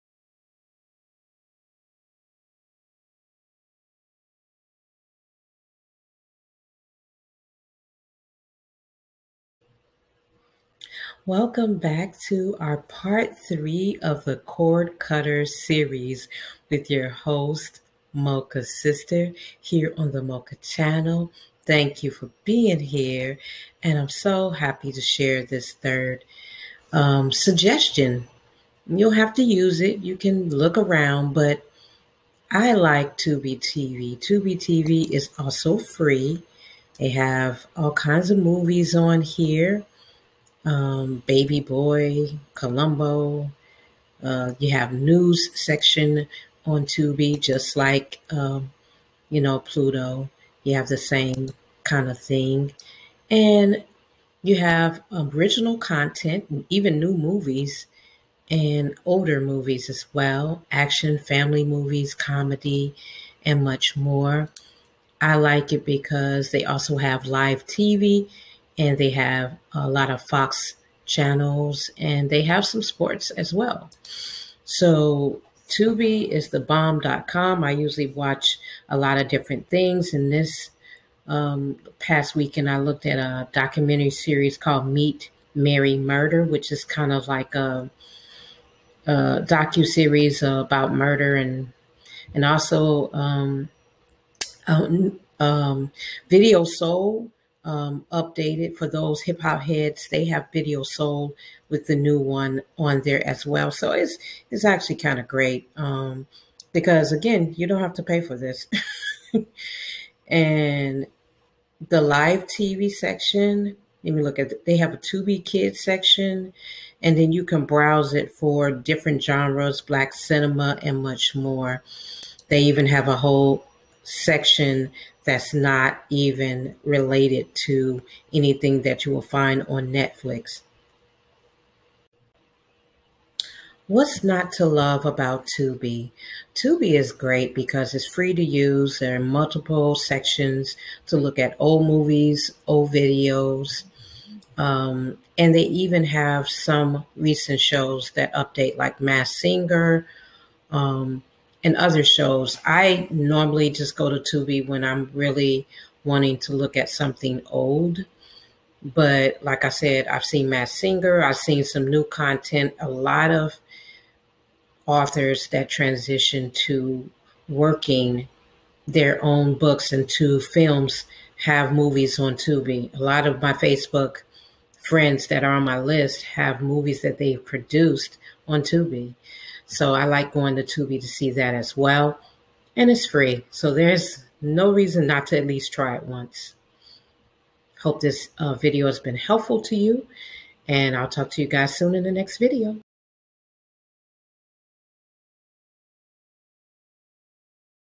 Cut cord series
In this episode, I cover another free streaming service called Tubi TV. You can download this on your smart tv, phone, use on your computer, apple tv and much more.

You will find many shows such as Masked Singer, The reboot of Video Soul with Donnie Simpson, various movies, and tv shows. This is completely free. There is no need to pay or use a credit card.

This short review is unsponsored and I highly suggest you at least try this as you cut the cord. Wishing you well.